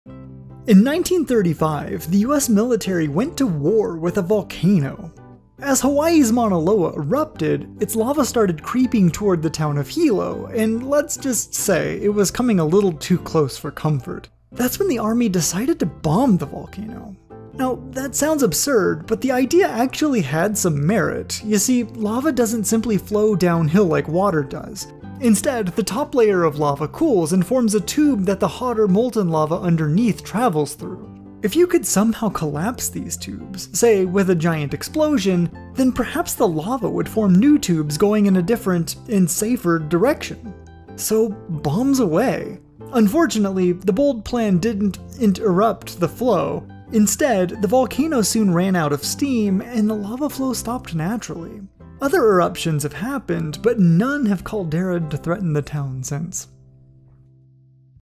When America went to War with a Volcano!
America once went to war with... a volcano?

When Mauno Loa in Hawaii erupted, the flow of lava crept towards the town of Hilo. The most straightforward way to stop it seemed to be by bombing it!
Luckily, the volcano ran out of steam before they had to take direct measures.